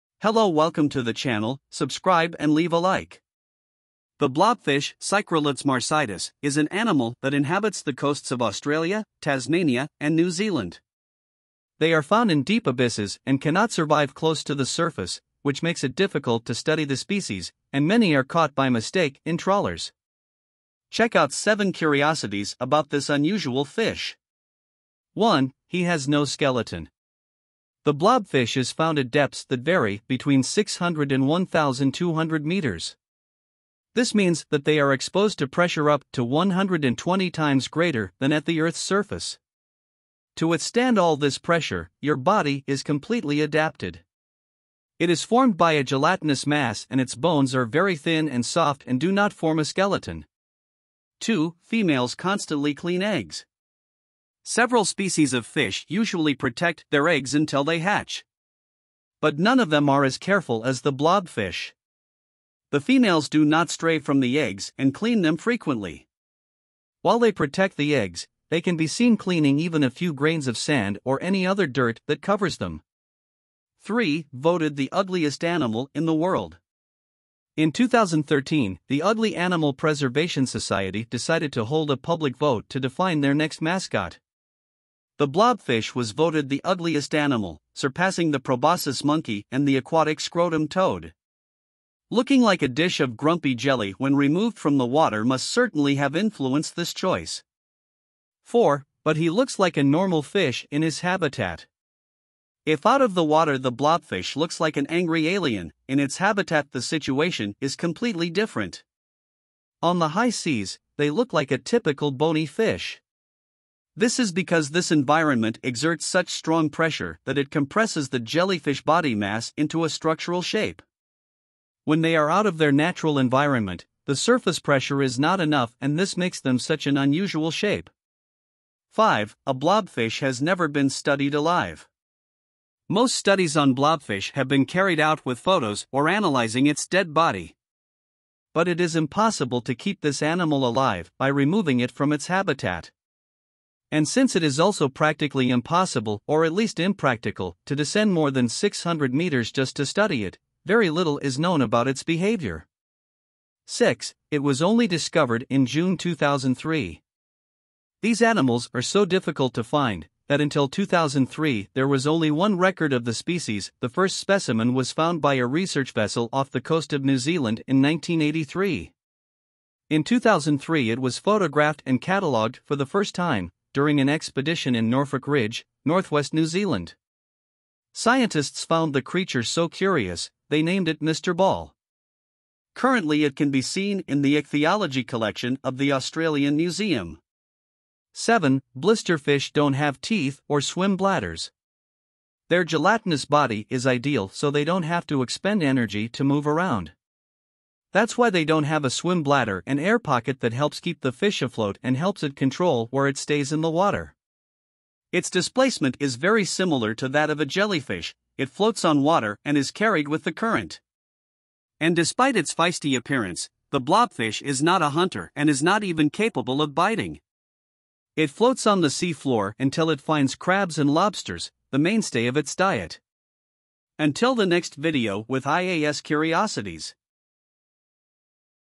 7 CURIOSITIES ABOUT THE BUBBLEFISH, THE UGLIEST ANIMAL IN THE WORLD-CURIOUS HEAD
Social curiosity is talking, listening and watching others to learn what they are thinking and doing. “Human beings are inherently social animals, and the most effective and efficient way to determine whether someone is friend or foe is to obtain information.”

7 CURIOSITIES ABOUT THE BUBBLEFISH, THE UGLIEST ANIMAL IN THE WORLD

The blobfish (Psychrolutes marcidus) is an animal that inhabits the coasts of Australia, Tasmania and New Zealand. They are found in deep abysses and cannot survive close to the surface, which makes it difficult to study the species, and many are caught by mistake in trawlers. Check out seven curiosities about this unusual fish.
1. He has no skeleton. The blobfish is found at depths that vary between 600 and 1,200 meters. This means that they are exposed to pressure up to 120 times greater than at the Earth's surface. To withstand all this pressure, your body is completely adapted. It is formed by a gelatinous mass and its bones are very thin and soft and do not form a skeleton.

2. Females constantly clean eggs. Several species of fish usually protect their eggs until they hatch. But none of them are as careful as the blobfish. The females do not stray from the eggs and clean them frequently. While they protect the eggs, they can be seen cleaning even a few grains of sand or any other dirt that covers them.

3. Voted the ugliest animal in the world. In 2013, the Ugly Animal Preservation Society decided to hold a public vote to define their next mascot. The blobfish was voted the ugliest animal, surpassing the proboscis monkey and the aquatic scrotum toad. Looking like a dish of grumpy jelly when removed from the water must certainly have influenced this choice.

4. But he looks like a normal fish in his habitat. If out of the water the blobfish looks like an angry alien, in its habitat the situation is completely different. On the high seas, they look like a typical bony fish. This is because this environment exerts such strong pressure that it compresses the jellyfish body mass into a structural shape. When they are out of their natural environment, the surface pressure is not enough and this makes them such an unusual shape.

5. A blobfish has never been studied alive. Most studies on blobfish have been carried out with photos or analyzing its dead body. But it is impossible to keep this animal alive by removing it from its habitat. And since it is also practically impossible — or at least impractical — to descend more than 600 meters just to study it, very little is known about its behavior.

6. It was only discovered in June 2003. These animals are so difficult to find, that until 2003 there was only one record of the species — the first specimen was found by a research vessel off the coast of New Zealand in 1983. In 2003 it was photographed and cataloged for the first time, during an expedition in Norfolk Ridge, northwest New Zealand. Scientists found the creature so curious, they named it “Mr. ball”. Currently it can be seen in the Ichthyology Collection of the Australian Museum.

7. Blisterfish don't have teeth or swim bladders. Their gelatinous body is ideal so they don't have to expend energy to move around. That's why they don't have a swim bladder — an air pocket that helps keep the fish afloat and helps it control where it stays in the water. Its displacement is very similar to that of a jellyfish, it floats on water and is carried with the current. And despite its feisty appearance, the blobfish is not a hunter and is not even capable of biting. It floats on the sea floor until it finds crabs and lobsters, the mainstay of its diet. until the next video with ias curiosities